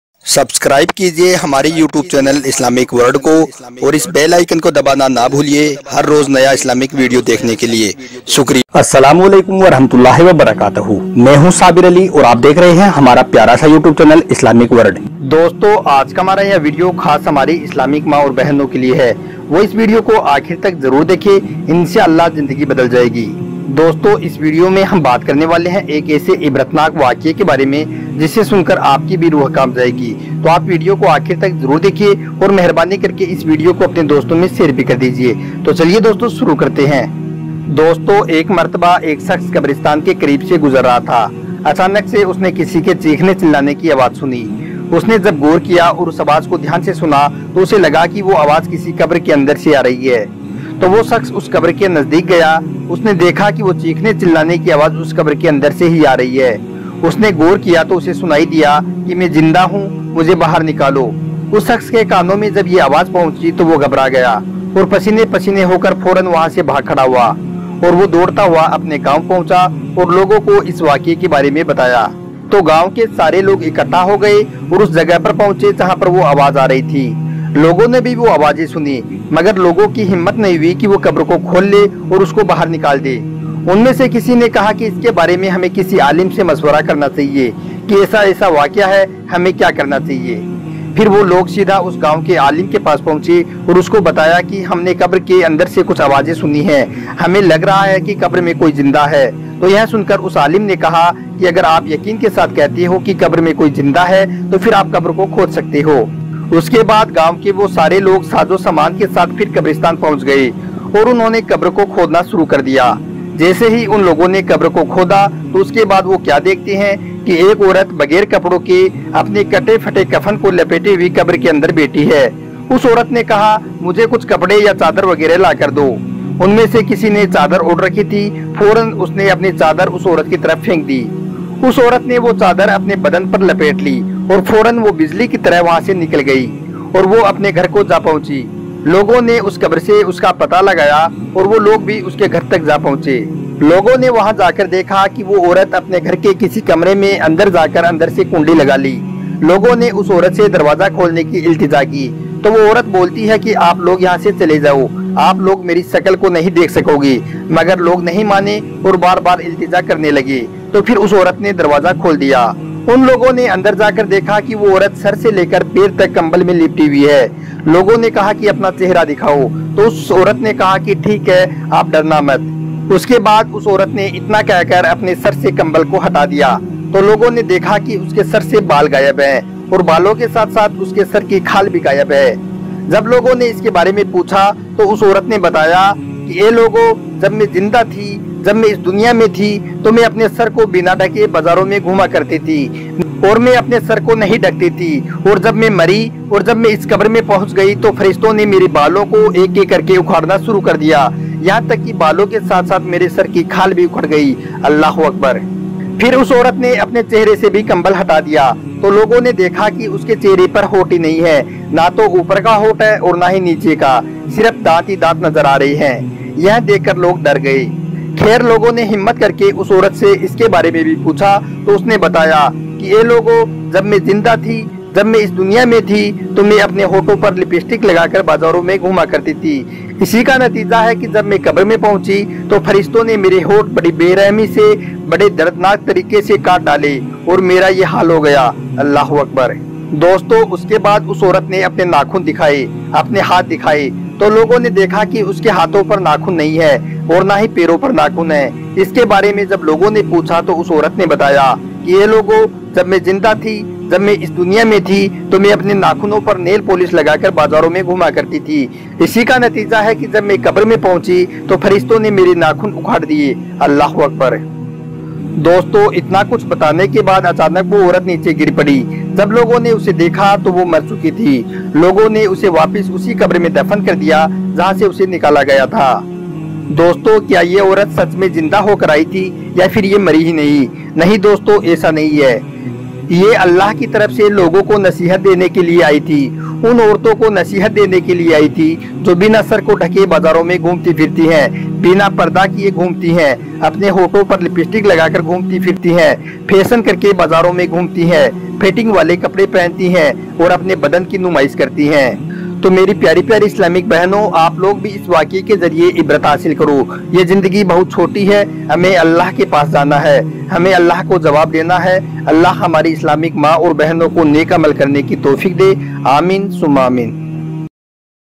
Allah Ki Qudrat - यह वीडियो हर मुस्लिम लड़की को एक बार जरूर देखना चाहिए
is video me aapko aik ladki ka esa ibratnak waqia bataya gaya hai jisme wo qabar se zinda aai uske baad m wo jo kuch batati ha usko sunkar yakinan aap bhi hairan rah jaaoge isliye yah video aapke liye aapke liye bahut khas hai ap isko aakhir tak zarur dekhe or mehrbani karke share bhi kar de taki aur bhi logo ke paas yah video pahuch jaye